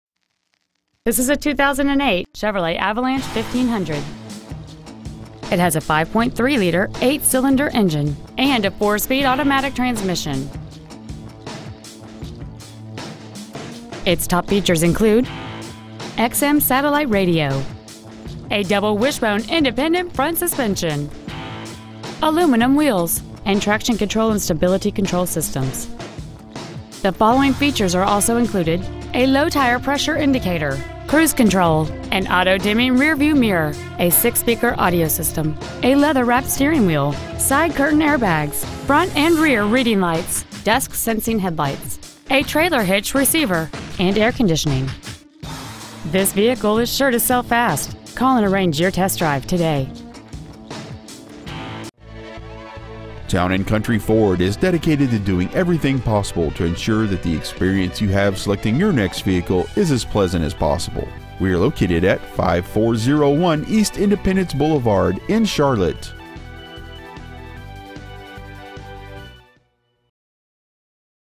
Pre-Owned 2008 CHEVROLET AVALANCHE Charlotte NC
We have been honored to serve the Charlotte NC area , we promise that your experience at our dealership will exceed your expectations ! Year : 2008 Make : CHEVROLET Model : AVALANCHE Engine : 5.3L V8 16V MPFI OHV FLEXIBLE FUEL Trans . : 4-SPEED AUTOMATIC Exterior : BLUE Miles : 103,431 Interior : GRAY Stock : T8G244118 Town and Country Ford Inc E Independnce Charlotte , NC 28212